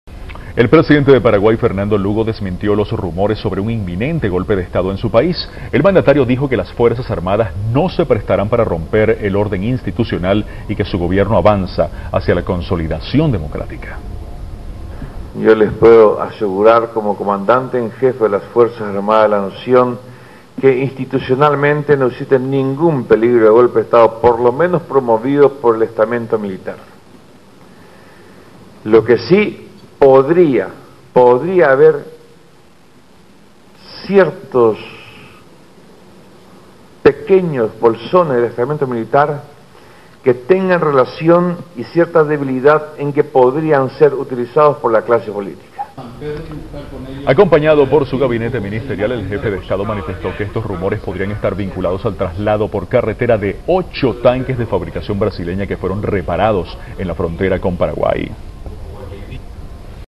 Fernando Lugo desmiente Golpe de Estado en Paraguay 04/11/2009
Fernando Lugo asegura que nadie lo moverá del Gobierno hasta finalizar su mandato

En declaraciones a la prensa, el mandatario paraguayo expresó que aunque su gobierno es ''amable, participativo y dispuesto a recibir críticas'' no es débil en sus acciones políticas por lo que nadie lo moverá del poder porque el pueblo lo eligió para ejercer hasta el 2013.